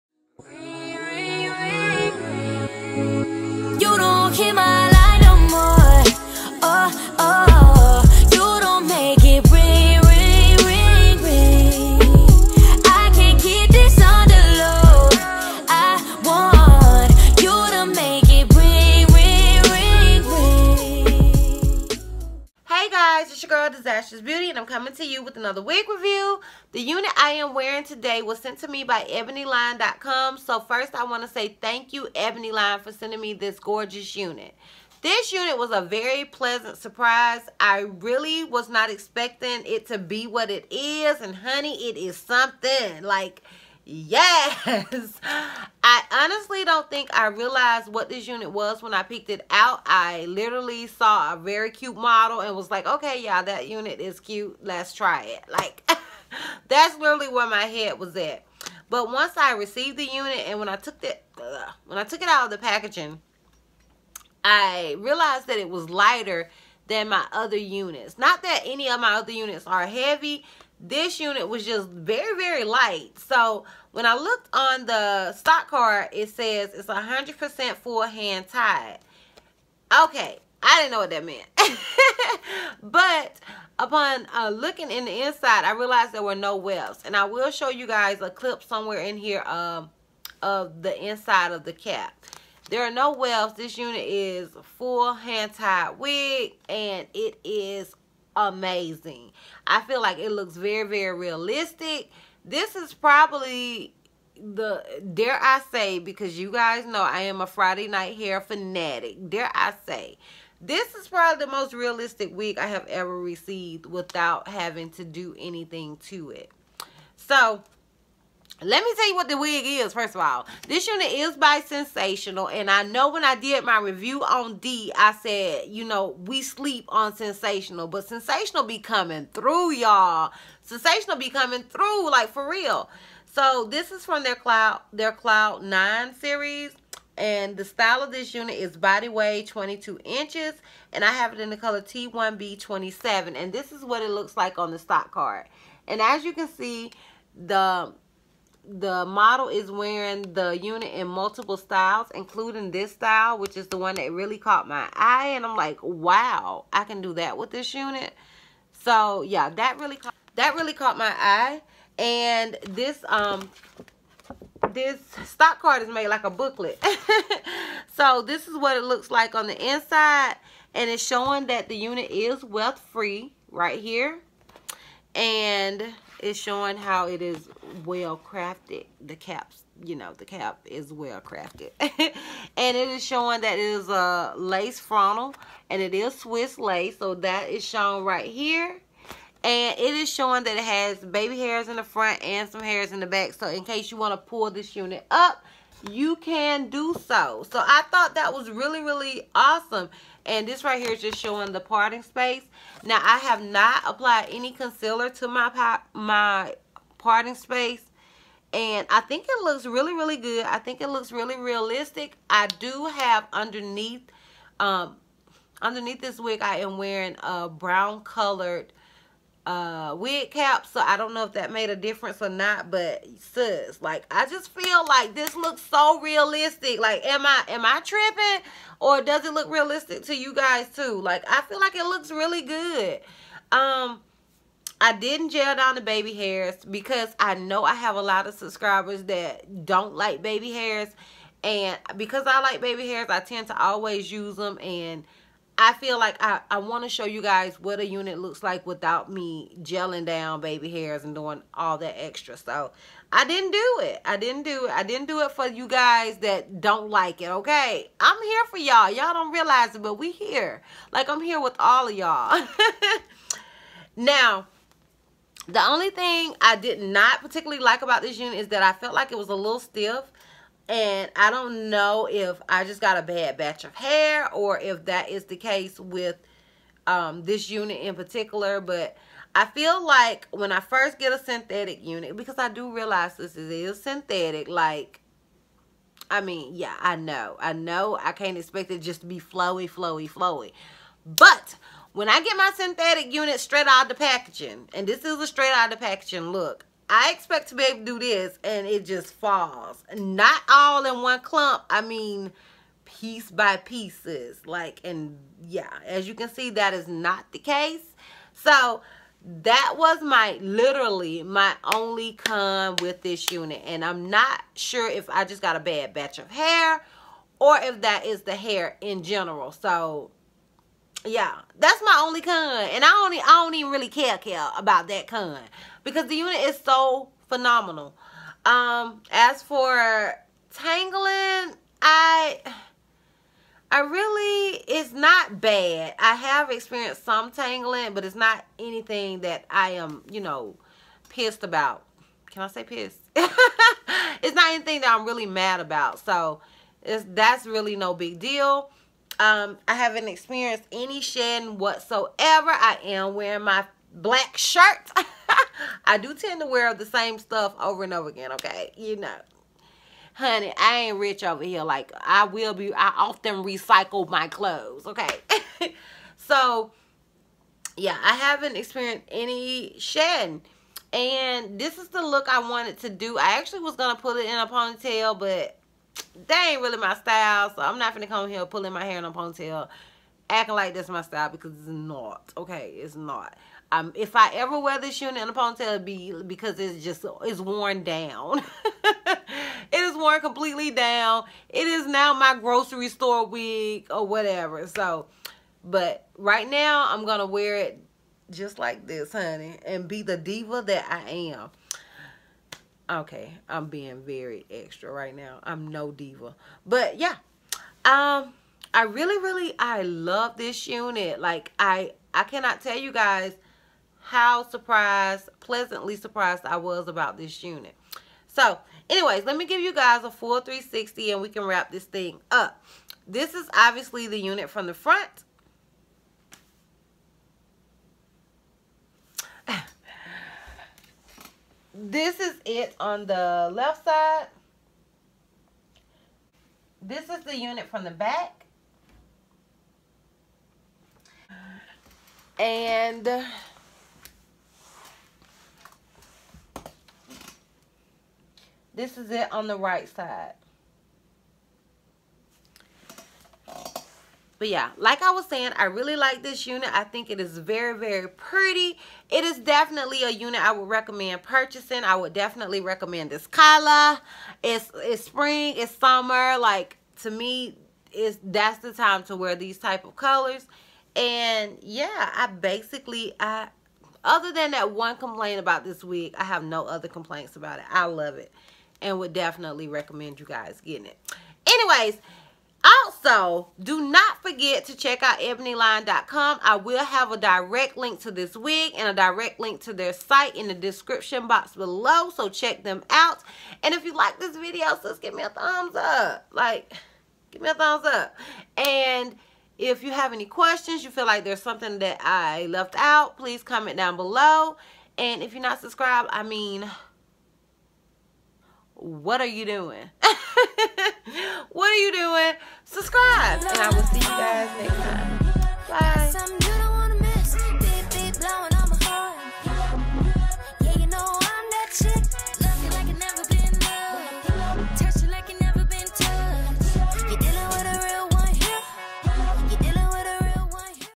FULL LACE SYNTHETIC WIG?!!| Sensationnel Cloud 9 Body Wave 22”| ft. Ebonyline.com
Hey Loves!!! This is my first full lace synthetic wig! This unit is by Sensationnel in the style Body Wave 22”. To find out my thoughts on this unit watch this full video😍😘 As usual thank u Loves for your love and support!!

Color: TT1B/27

IG&FB: dizastrousbeauty
SC: hail2mzc